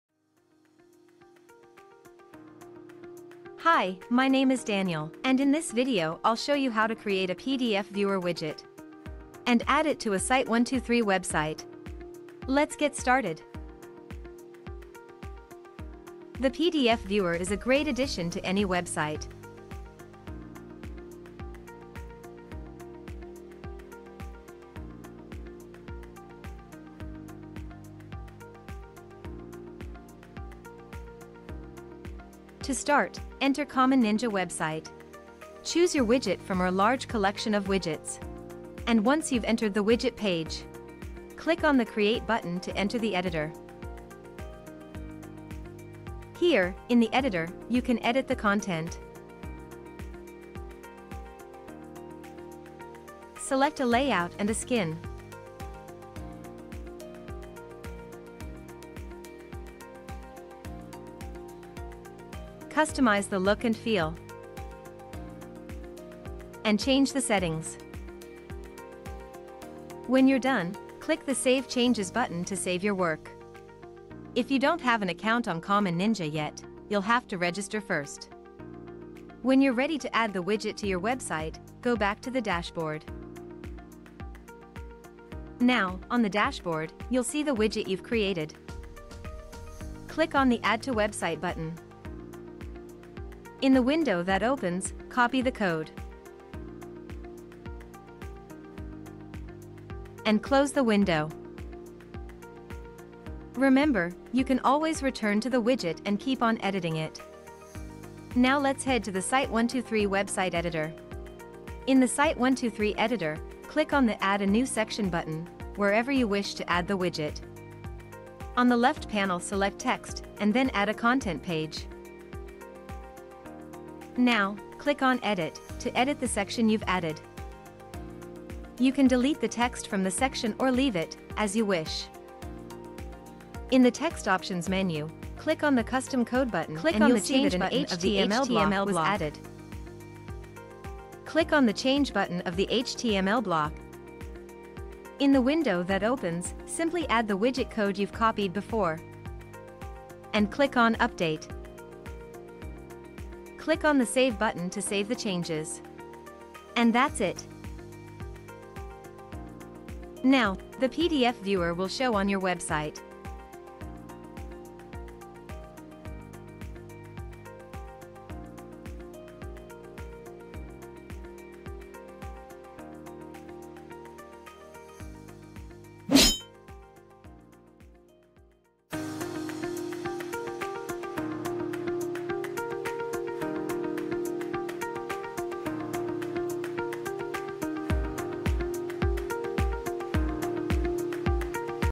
How to add a PDF Viewer to Site123
Common Ninja’s PDF Viewer is a powerful widget that allows users to view and download PDF files easily without leaving your site.
With it you can engage your audience, promote your content and offer a better user experience by providing helpful information. Make your ebooks available to view and download; provide your users with support documentation to your products and services; add manuals, journals, magazines, and any other type of information that’s relevant and can be converted to a PDF format — the options are limitless!

Video Timeline
00:00 About the PDF Viewer widget
00:32 Creating PDF Viewer widget
00:44 Editing the PDF Viewer widget
01:24 Adding the PDF Viewer widget to Site123

The PDF Viewer widget for Site123 has a wide variety of customizable features, including:
- Rich design options
- Multiple layouts
- Fully responsive